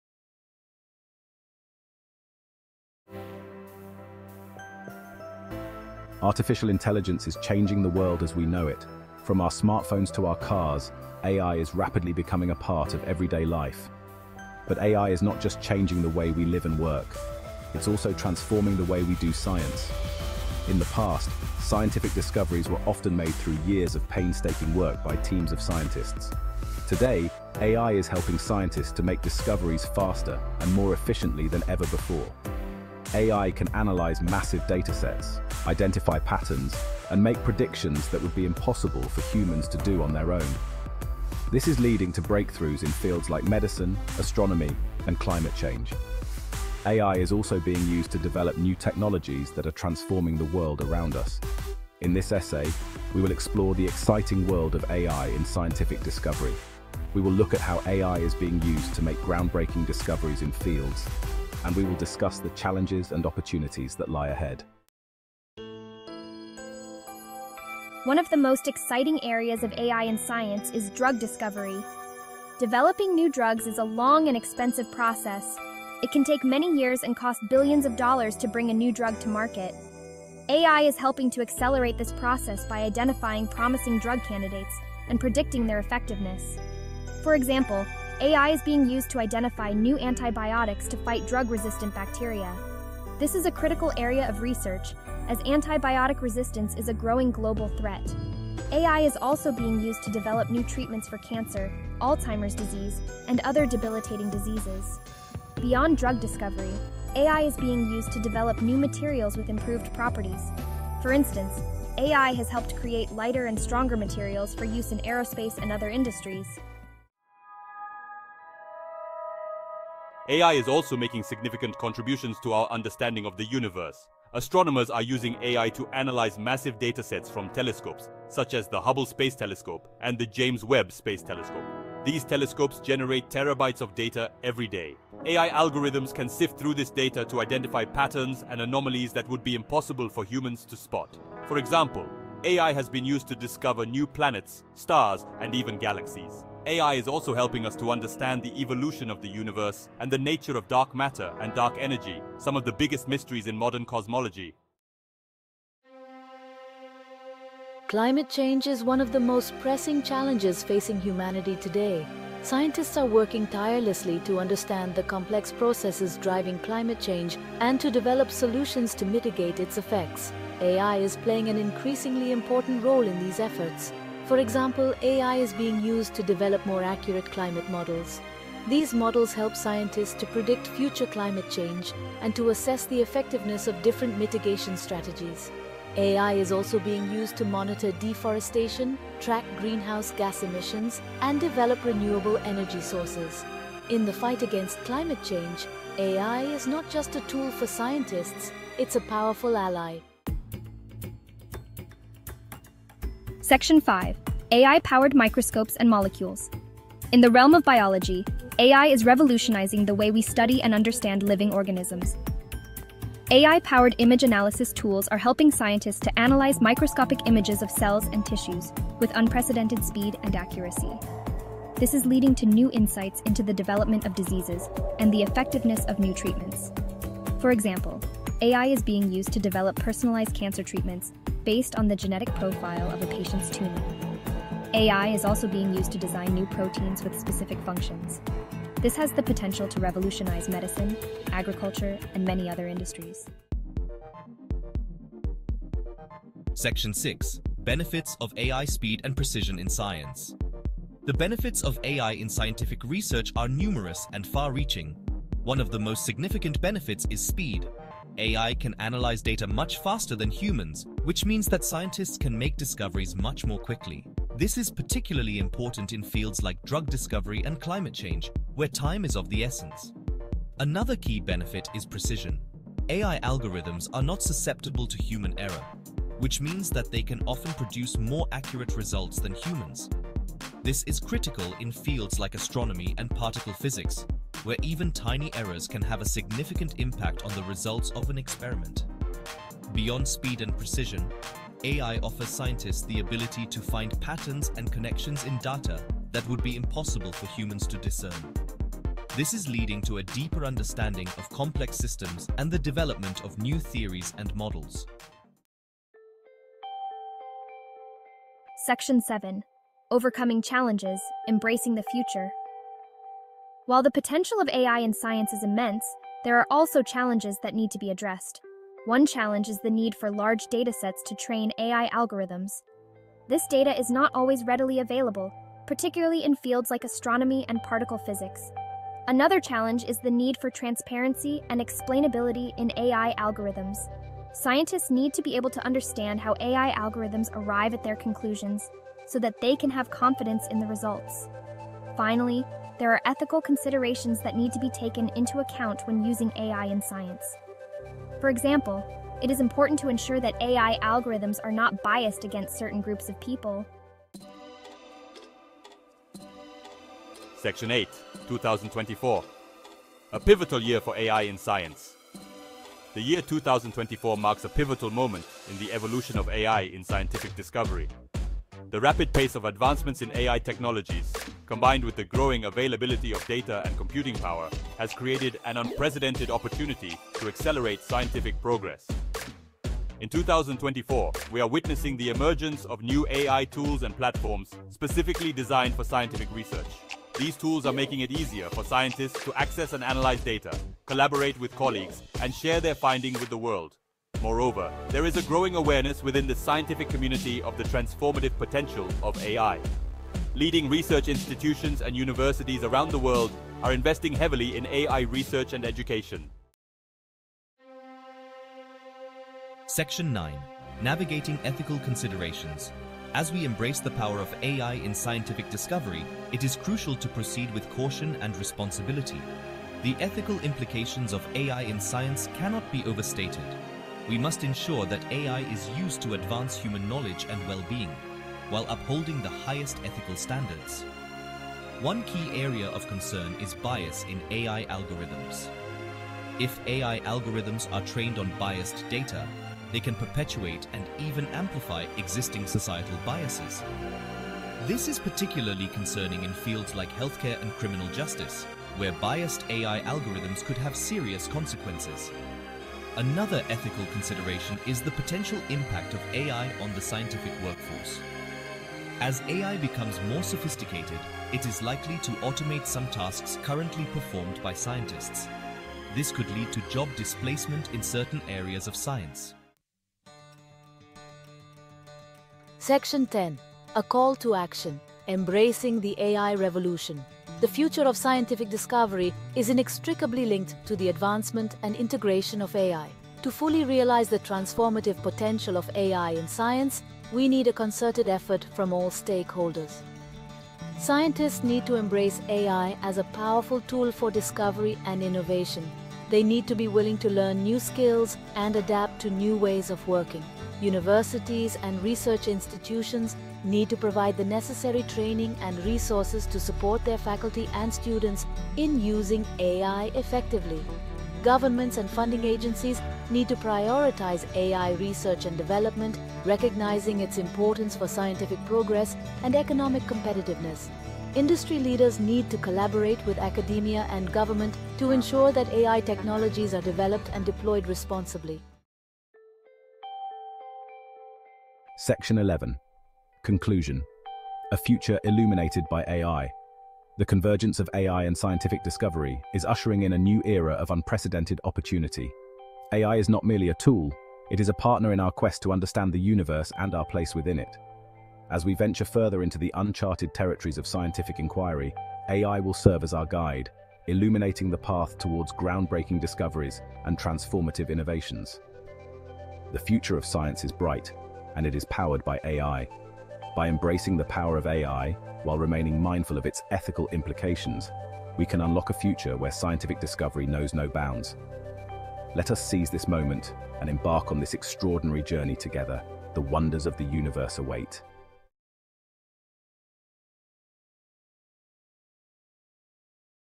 AI in 2024:Transforming Scientific Discovery and Research#ai
AI in 2024:Transforming Scientific Discovery and Research

AI for Scientific Discovery: Revolutionizing Research in 2024

🌟 Dive into the cutting-edge world of AI-driven scientific research in our latest video, "AI for Scientific Discovery: Revolutionizing Research in 2024." Discover how breakthroughs in artificial intelligence are accelerating discoveries across multiple fields, from biology and medicine to climate science and materials engineering. 🌐🔬

In this video, we explore:

How AI systems like DeepMind’s AlphaFold are revolutionizing our understanding of protein structures.

The role of AI in discovering new antibiotics and creating efficient battery materials.

The ethical and security considerations surrounding the use of AI in research.

Join us for a journey through the latest advancements and see how AI is shaping the future of science. Whether you're a tech enthusiast, a researcher, or just curious about the future, this video is a must-watch. 🌍💡